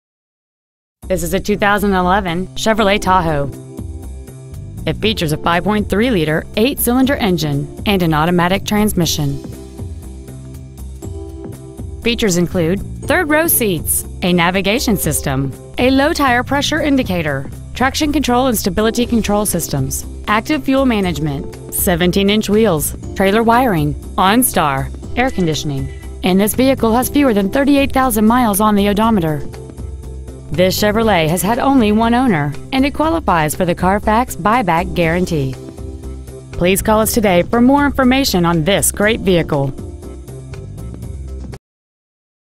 2011 Chevrolet Tahoe Grapevine TX 76051
Year : 2011
 Make : Chevrolet
 Model : Tahoe
 Engine : Vortec 5.3L V8 SFI FlexFuel
 Trans . : Automatic
 Exterior : Gray
 Miles : 37,040
 Interior : -

 801 State Hwy. 114 East